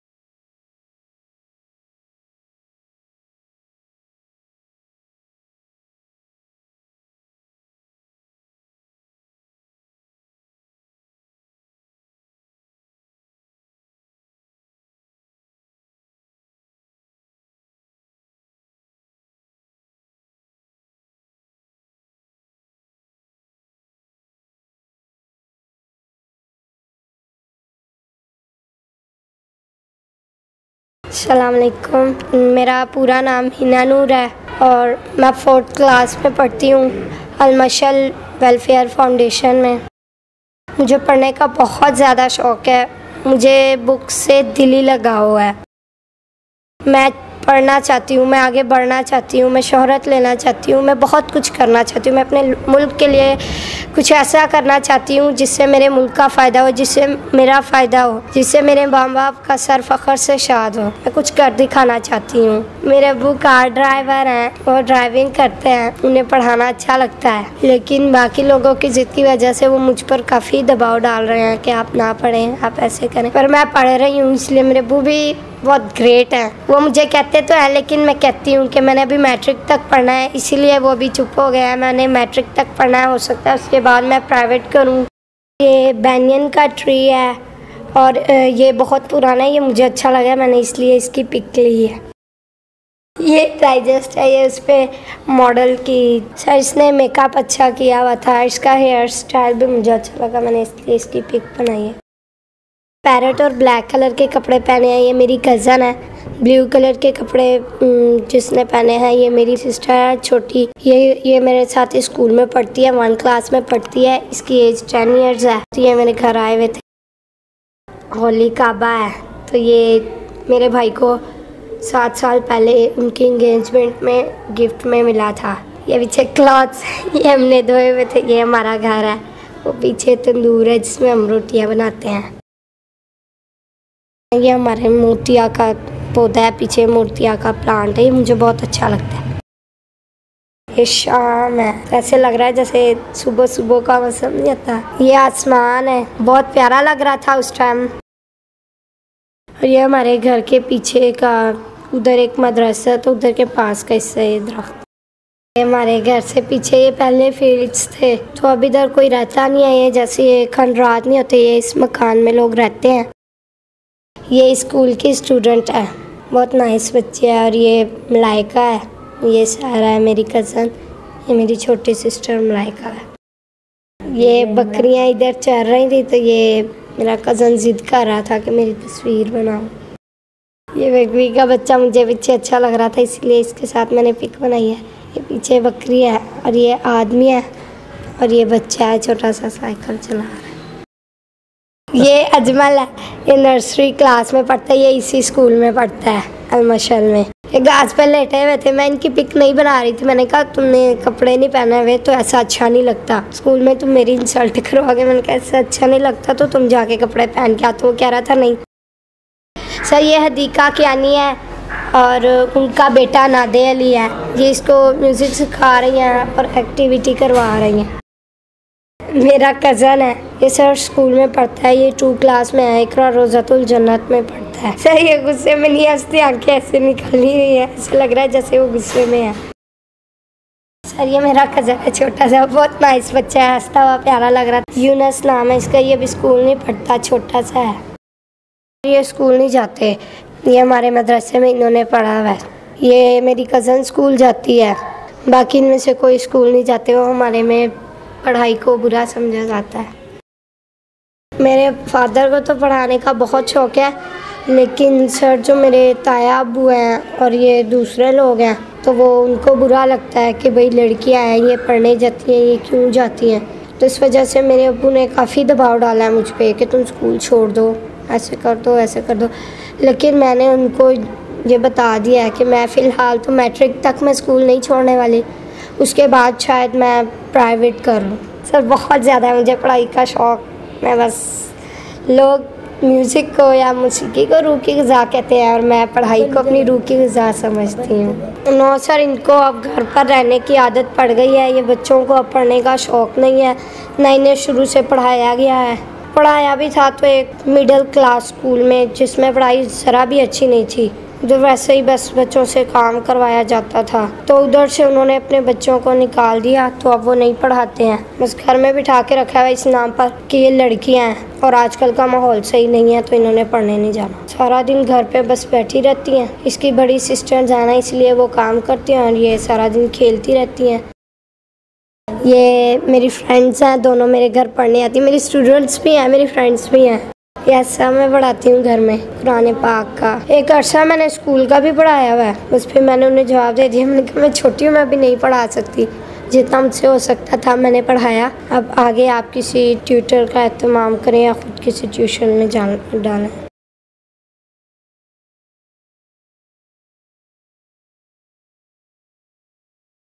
Camera Kahani - Hina Noor
Camera Kahani
Children documenting their own stories

The Little Art conducted a photography workshop with the children in marginalized schools. Children documented the stories of their life and what inspires them.
This is one of the story.